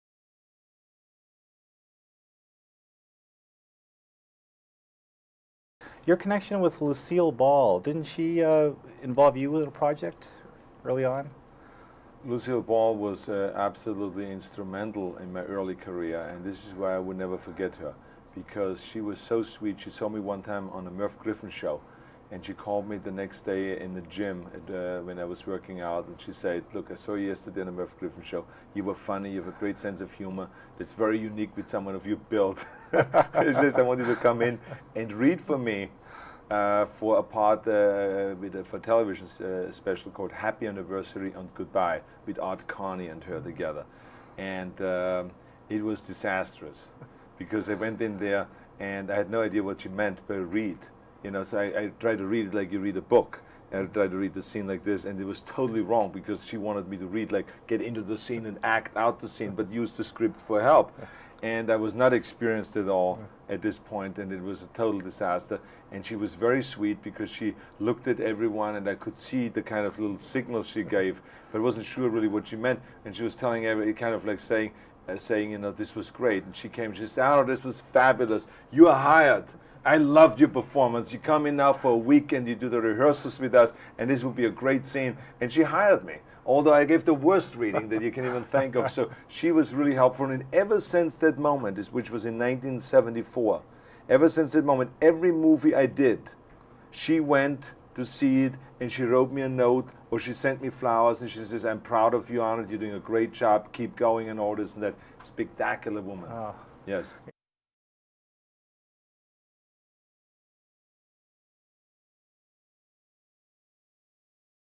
Rewind: Lucille Ball and Arnold Schwarzenegger
Back in 1996, I interviewed Arnold Schwarzenegger who told me the story of how Lucille Ball was instrumental in his early acting career.